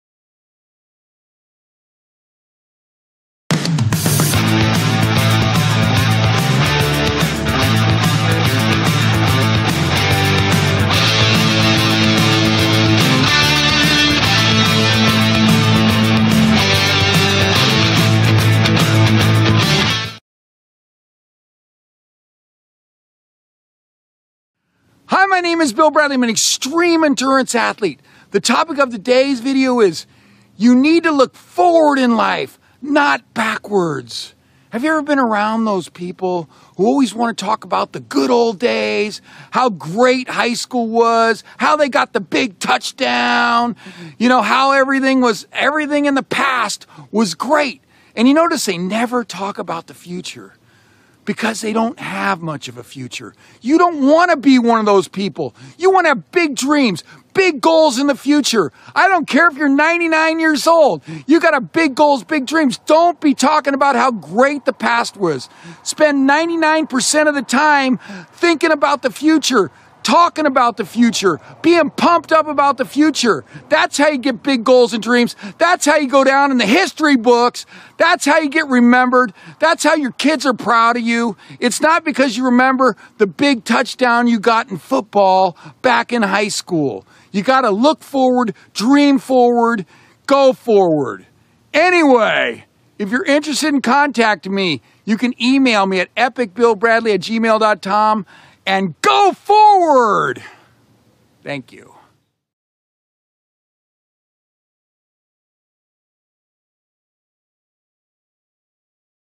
You Need To Look Forward In Life, Not Backwards!
Extreme endurance athlete & motivational speaker, Bill Bradley says "Look Forward, Dream Forward, Go Forward!"